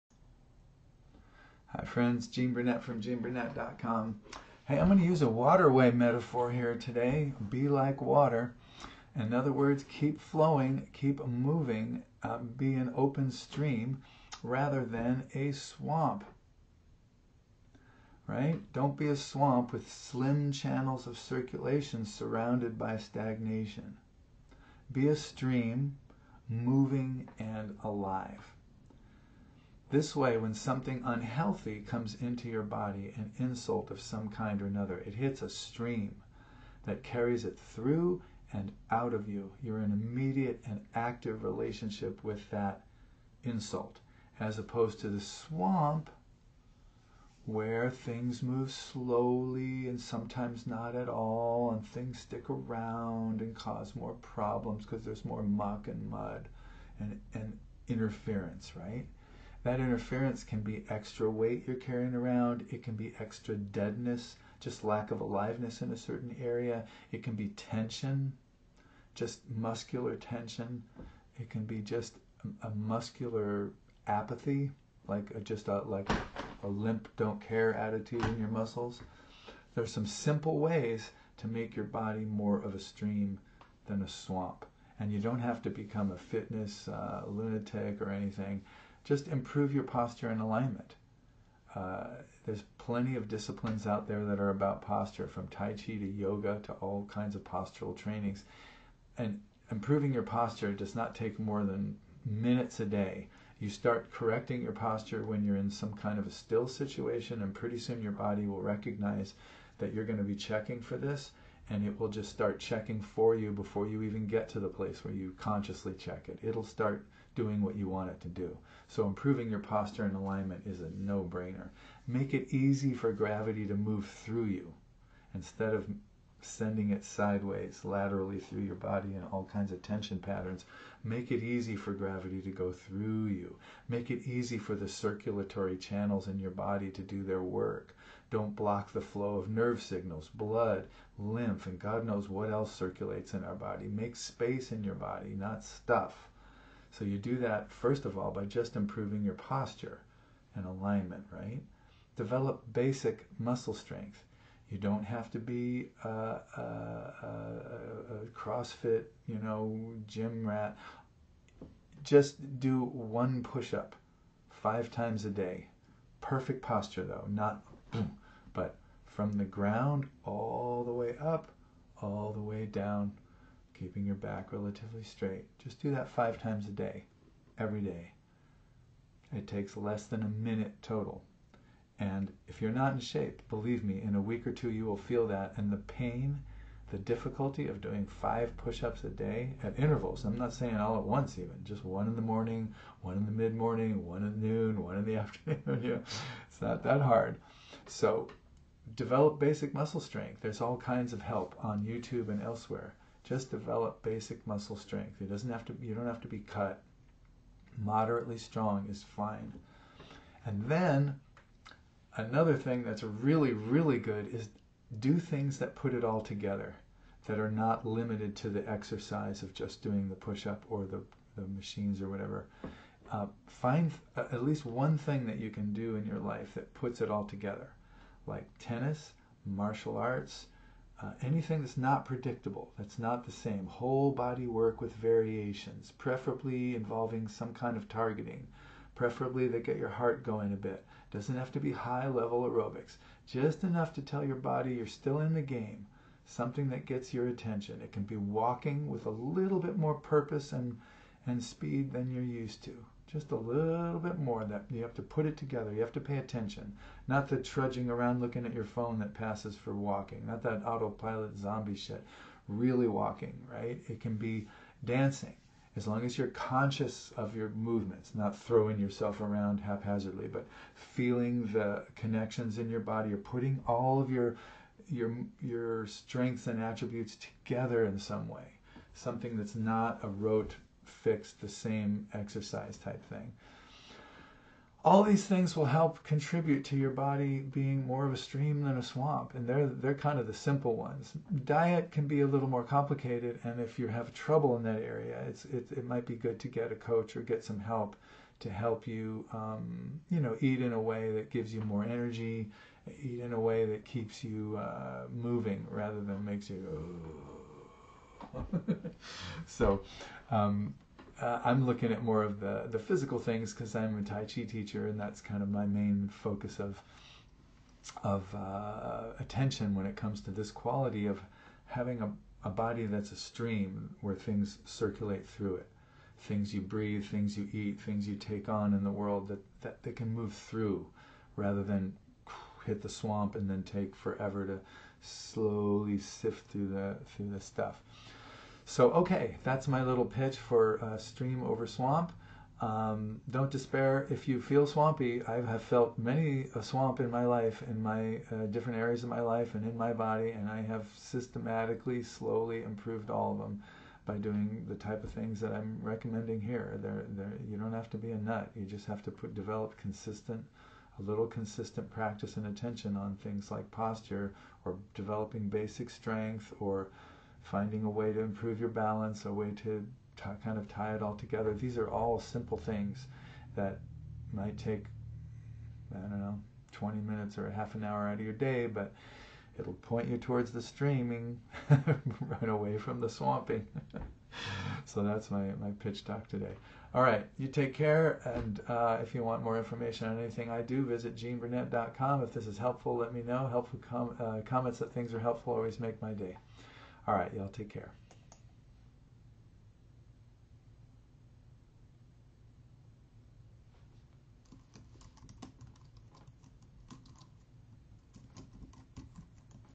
Stream vs. Swamp~Gene Burnett
Here I am talking about ways to make your body more of a stream internally and less of a swamp. A place of space and circulation rather than obstruction and stagnation.